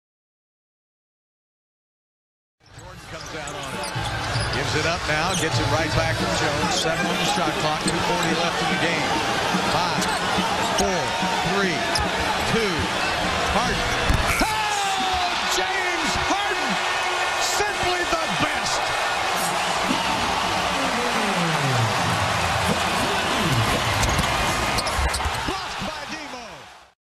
James Harden's stepback three dagger on JJ Redick
In the final few minutes of the game, James Harden hits a stepback three-pointer on JJ Redick that all but puts the game out of reach in the Houston Rockets 110-105 win over the Los Angeles Clippers on 2/25/2015.